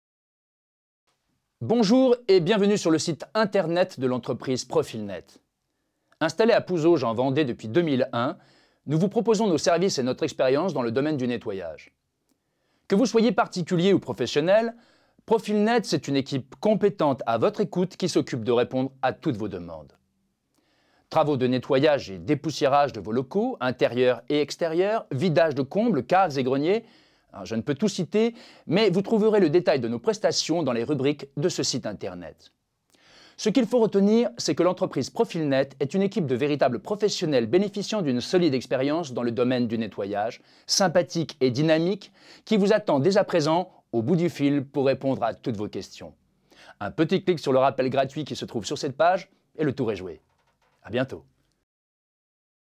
PROFIL'NETT : entreprise de nettoyage à Pouzauges 85
Bonjour et bienvenue sur le site internet de l'entreprise Profil'NETT ! Installés à Pouzauges en Vendée depuis 2001, nous vous proposons nos services et notre expérience dans le domaine du nettoyage. Que vous soyez particulier ou professionnel, Profil'NETT, c'est une équipe compétente à votre écoute qui s'occupe de répondre à toutes vos demandes.